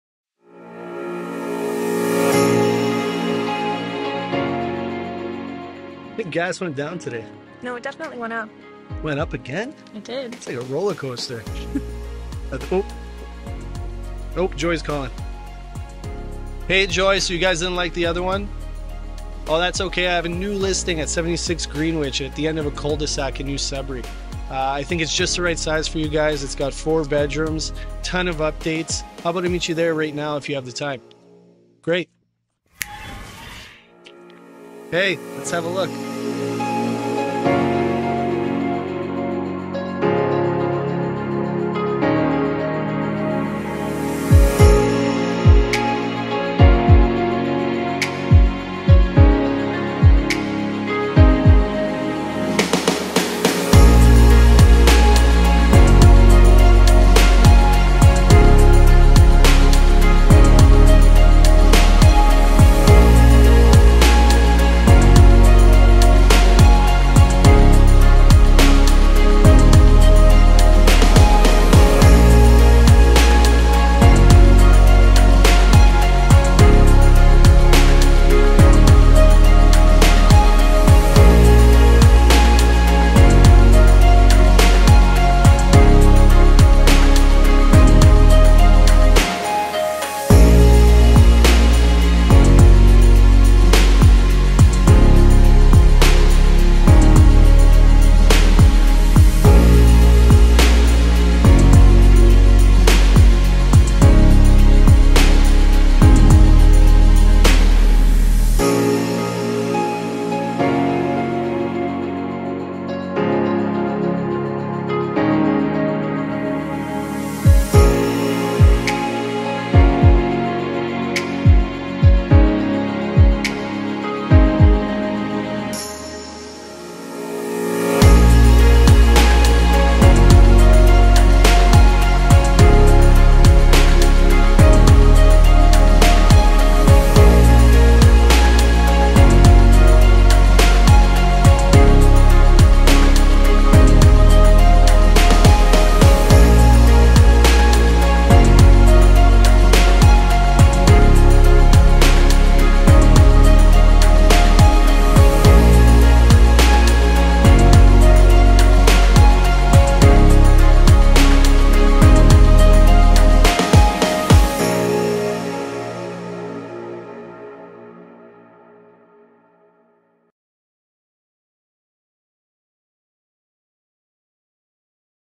Welcome to 76 Greenwich!!
This one has it all! Welcome to 76 Greenwich, sitting at the end of a cul-de-sac in one of New Sudbury's finest areas. Walking in, your entertained with a large tiled entrance, leading to the very spacious living room. This floor also has a half bathroom and laundry area. Making your way to the next level, you can't miss the completely updated kitchen area with quartz countertops, opened all the way through the bar and dining room area. Making your way upstairs, you have 3 bedrooms and 2 full bathrooms. One bathroom being an ensuite off the spacious master bedroom. Stay a while and enjoy the recently installed steam shower! Downstairs offers a 3rd full bathroom, extra office area, tons of storage space and a walkout to the backyard. Outside offers a newer lock stone driveway leading to the double garage. The backyard is very private, fully-fenced and comes with a large 20x30, newly-built deck! This home also has multiple cameras on the property. You won't want to miss your chance to see this one!!